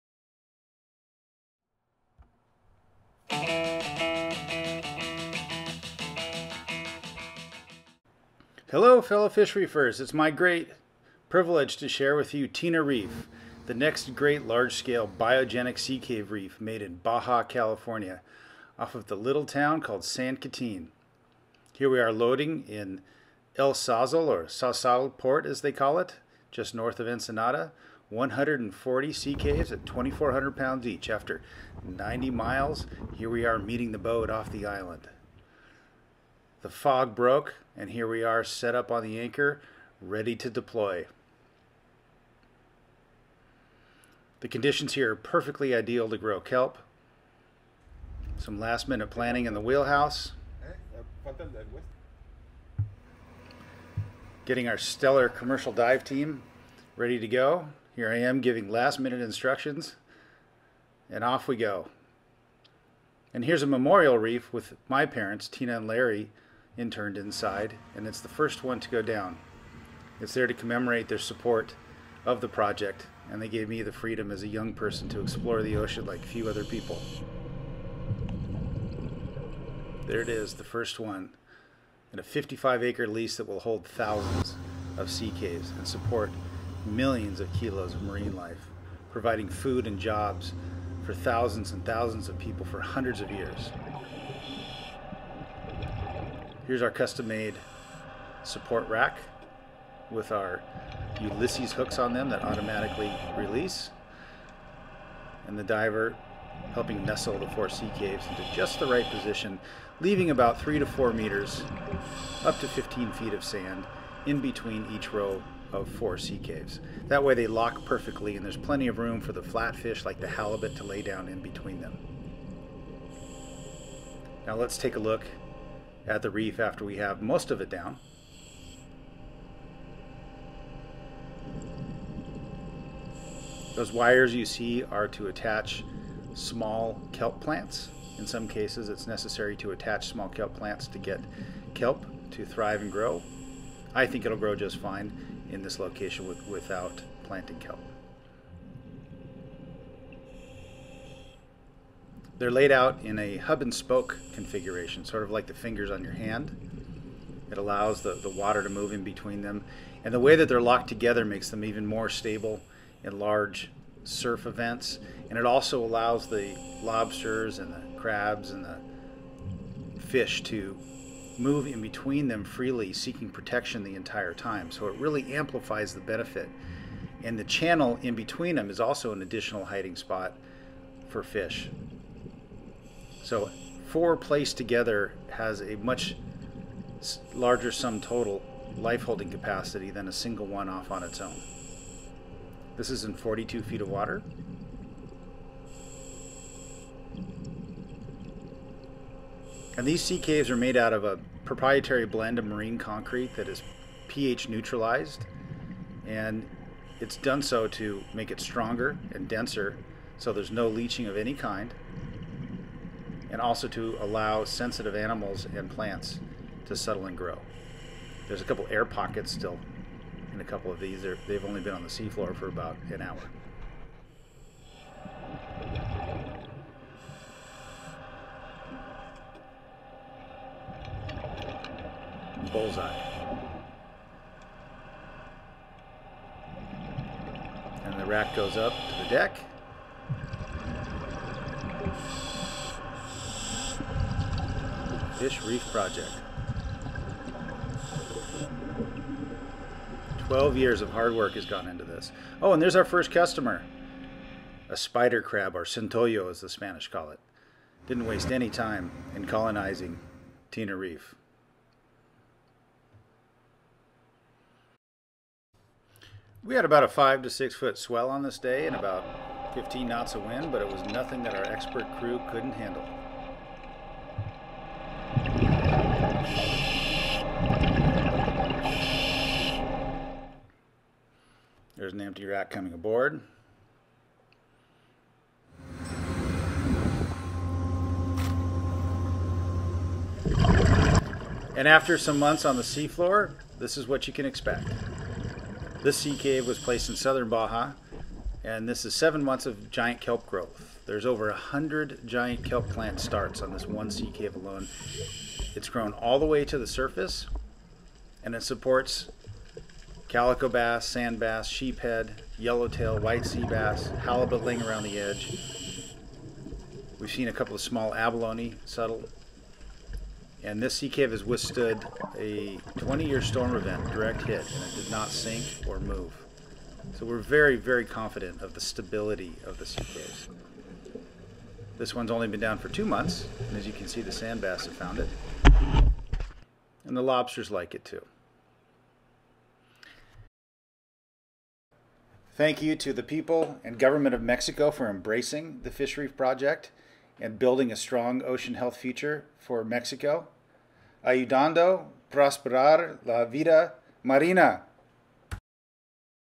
Tina Reef- massive Sea Cave Reef deployed off Baja Mexico
Baja and the West Coast's first large -scale  biogenic reef is deployed in ideal kelp growing region.